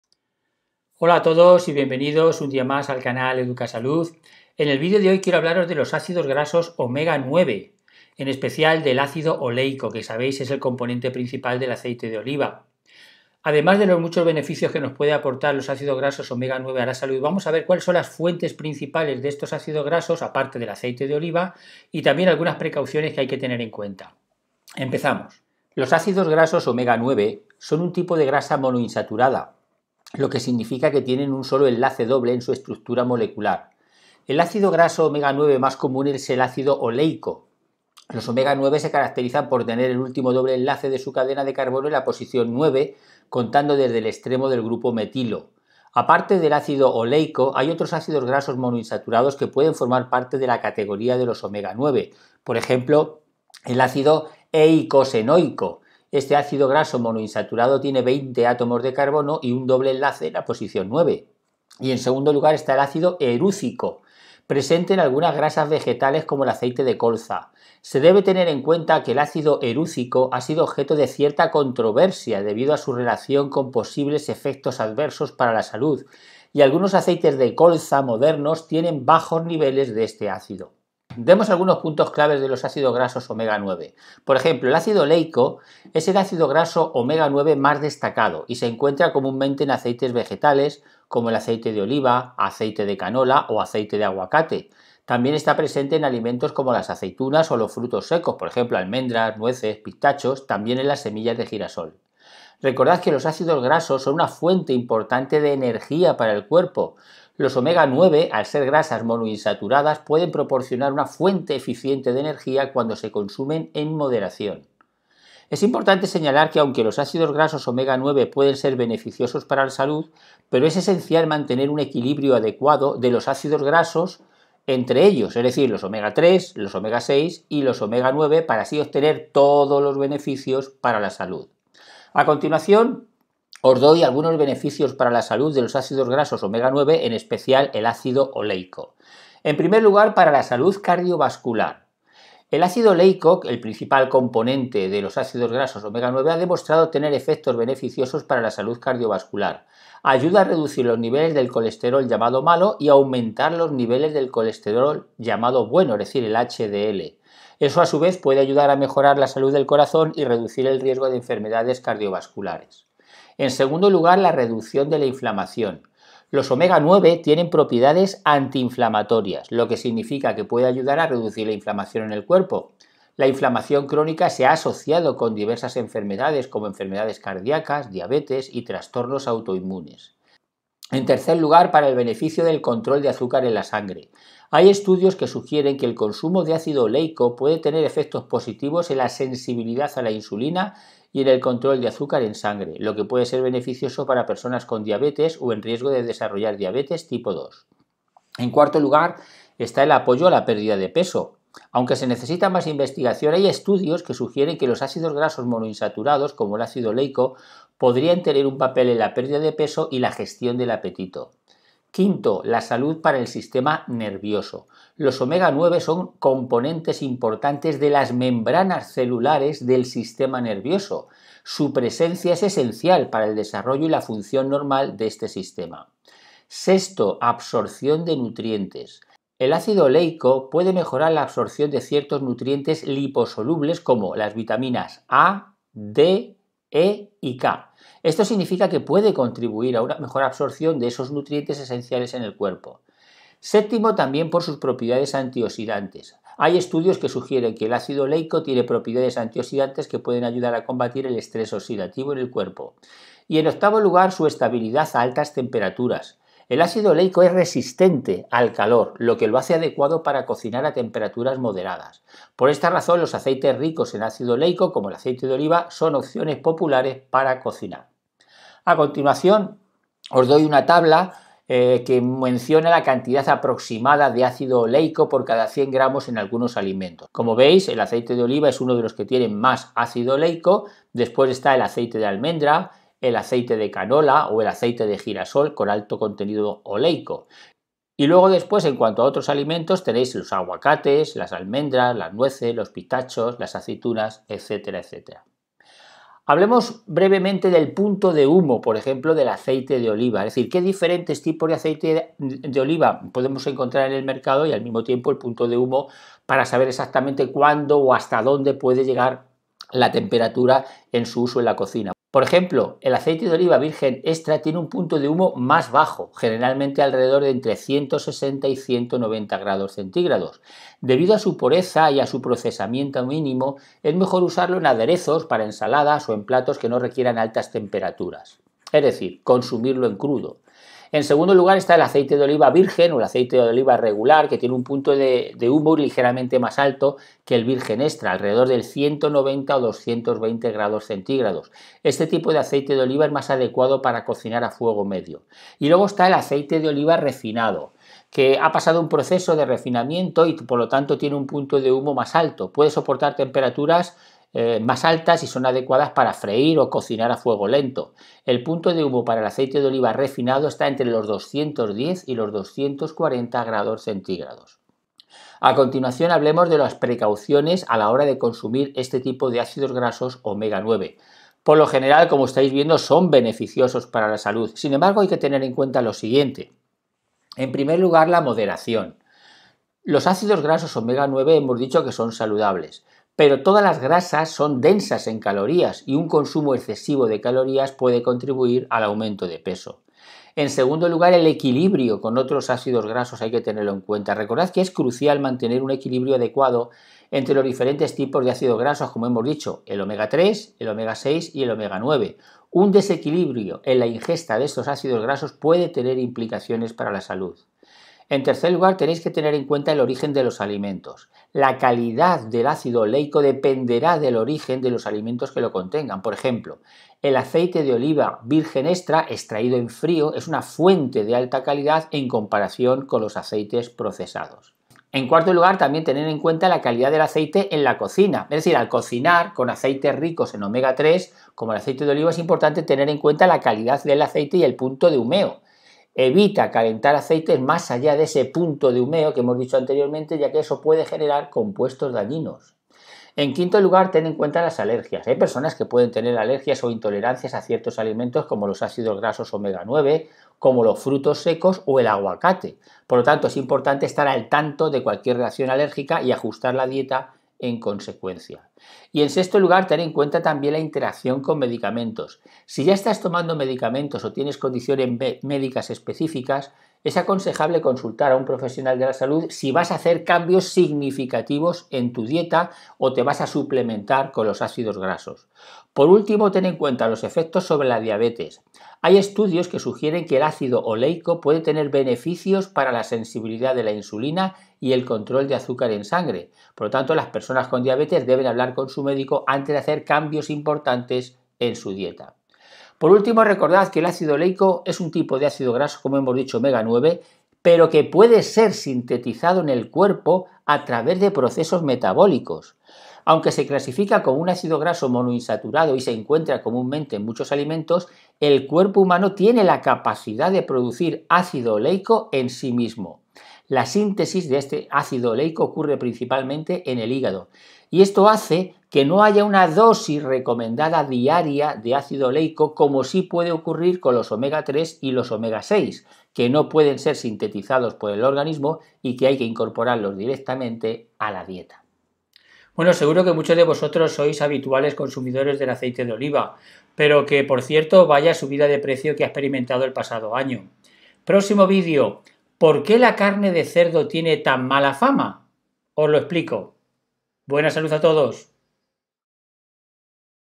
ÁCIDOS GRASOS OMEGA 9 - Ácido Oleico: Propiedades y Beneficios
Los ácidos grasos omega-9 son un tipo de grasa monoinsaturada, lo que significa que tienen un solo enlace doble en su estructura molecular. El ácido graso omega-9 más común es el ácido oleico.
Otros ácidos grasos omega 9: Ácido Eicosenoico y Ácido Erúcico.
Beneficios para la salud de los ácidos grasos omega 9.
-Salud cardiovascular.
-Reducción de la inflamación.
-Control de azúcar en sangre.
-Apoyo a la pérdida de peso.
-Salud del sistema nervioso.
-Absorción de nutrientes.
-Propiedades antioxidantes.
-Estabilidad a altas temperaturas.

Principales alimentos ricos en ácidos grasos omega 9.
Precauciones a tener en cuenta con el consumo de ácidos grasos omega 9 y ácido oleico.